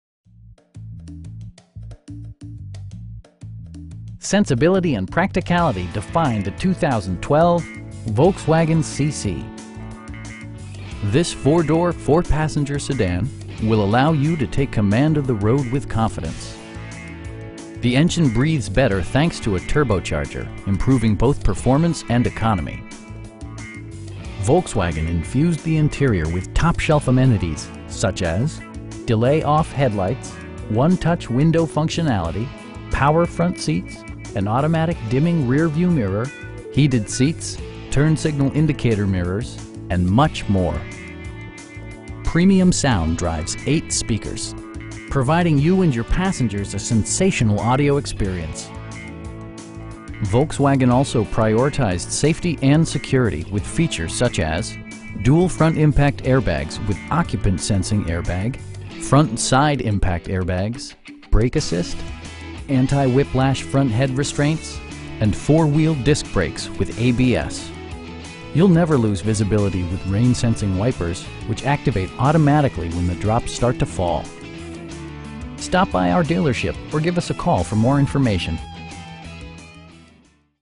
2012 Volkswagen CC in Orlando, FL 32810
David Maus VW North
1050 Lee Road

Sensibility and practicality define the 2012 Volkswagen CC. This 4 door, 4 passenger sedan will allow you to take command of the road with confidence! The engine breathes better thanks to a turbocharger, improving both performance and economy. Volkswagon infused the interior with top shelf amenities, such as: delay-off headlights, 1-touch window functionality, power front seats, an automatic dimming rear-view mirror, heated seats, turn signal indicator mirrors, and much more. Premium sound drives 8 speakers, providing you and your passengers a sensational audio experience. Volkswagon also prioritized safety and security with features such as: dual front impact airbags with occupant sensing airbag, front SIDE impact airbags, traction control, brake assist, anti-whiplash front head restraints, a security system, and 4 wheel disc brakes with ABS. You'll never lose visibility with rain sensing wipers, which activate automatically when the drops start to fall. We pride ourselves in consistently exceeding our customer's expectations. Stop by our dealership or give us a call for more information.